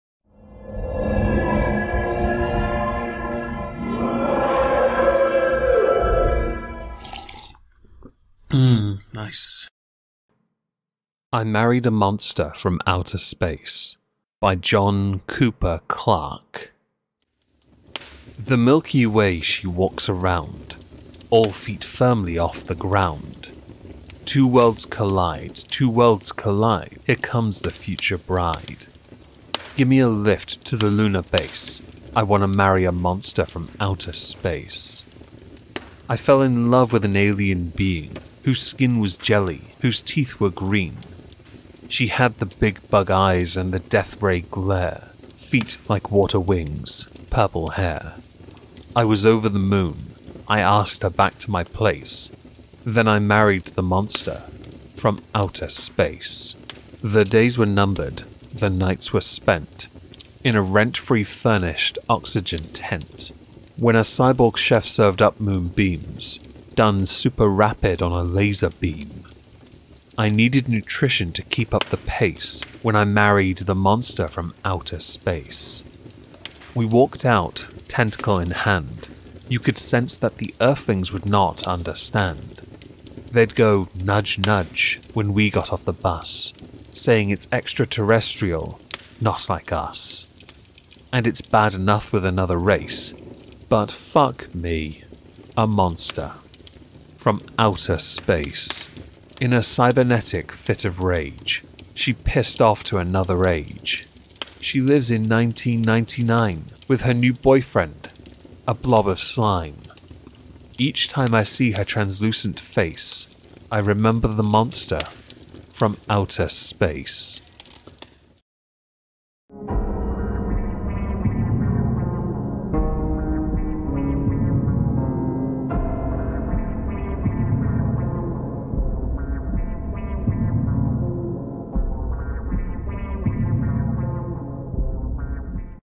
(I Married A) Monster From Outer Space by John Cooper Clarke
Welcome back to Moistski Reads! Today, we shall be delving into the works of John Cooper Clarke.